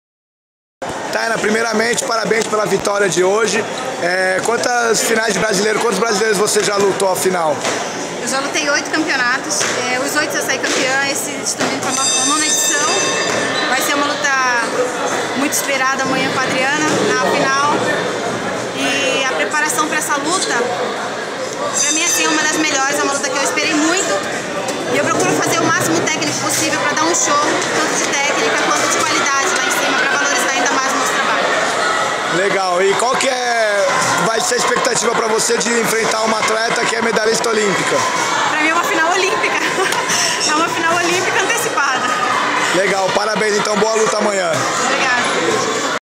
Taynna Cardoso fala sobre sua luta com Adriana Araújo
Taynna Cardoso fala sobre sua luta de amanhã com Adriana Araújo, pela final do brasileiro feminino 2014.